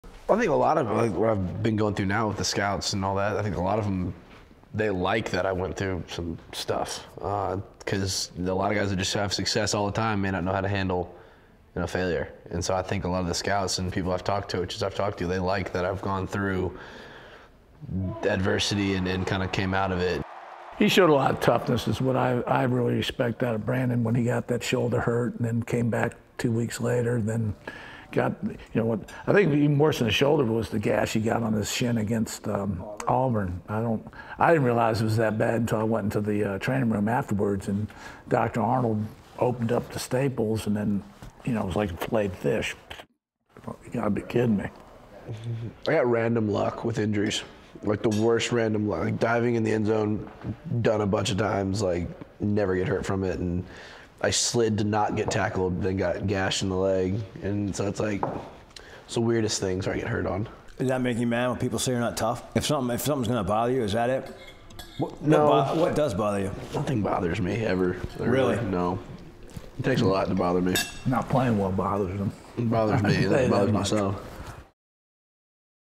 The Allens: Brandon & Bobby Allen talk scouting and the draft
See how adversity helped Brandon Allen prove himself to NFL teams while father Bobby explains the one thing that really bothers him.